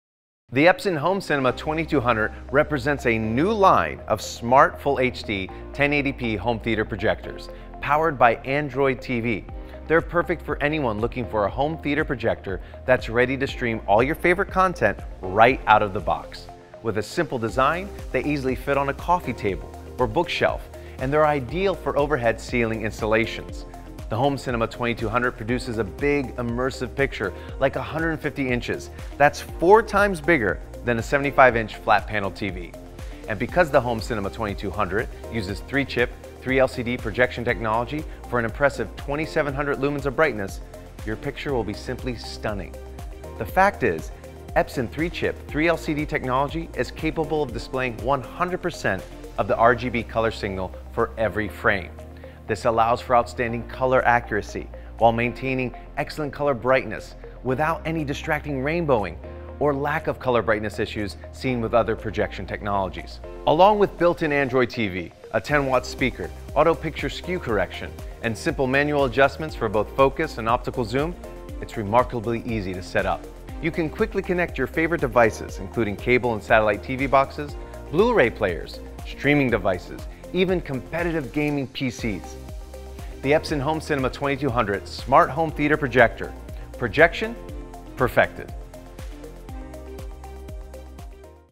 Epson Home Cinema 2200 Smart 3-Chip 3LCD Projector with Android TV
The Epson Home Cinema 2200 displays vivid content with Best-in-Class Color Brightness with unique 3-Chip 3LCD technology. The perfect choice for streaming TV shows, sporting events, movies and more, this dynamic projector features Image Enhancement for smooth, crisp images and accepts 4K – for an astounding Full HD picture. Plus, with built-in Android TV2 and wireless connectivity, you get seamless access to popular streaming services including Hulu, HBO and YouTube™, right out of the box.